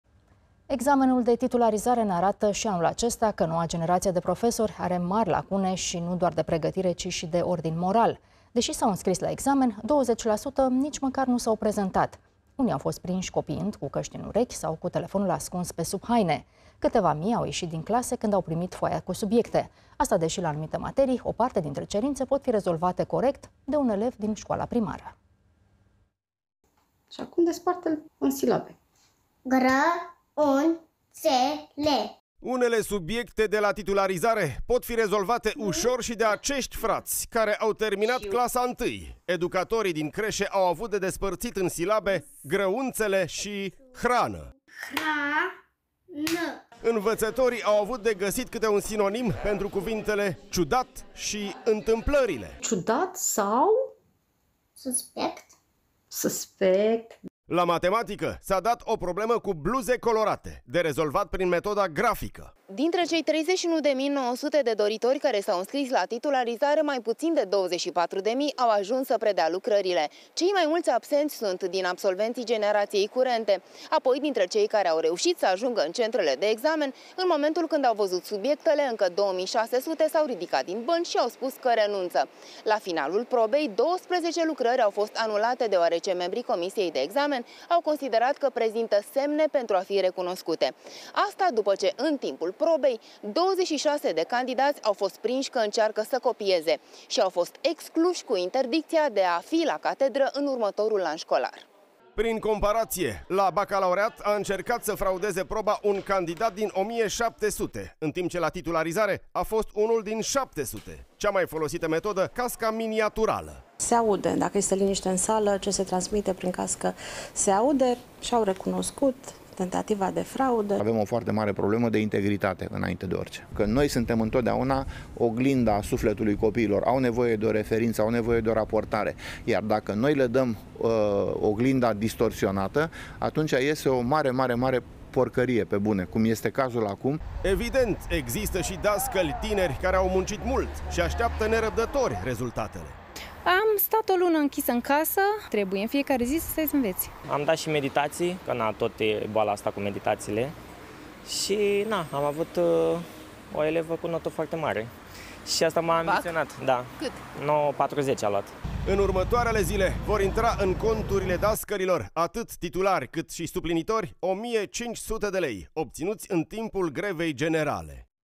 De trei ori mai mulți profesori care au copiat la titularizare decât elevi la bac
Examenul de titularizare ne arată, şi anul acesta, că noua generaţie de profesori are mari lacune, şi nu doar de pregătire, ci şi de ordin moral. Deși s-au înscris la examen, 20% nici măcar nu s-au prezentat.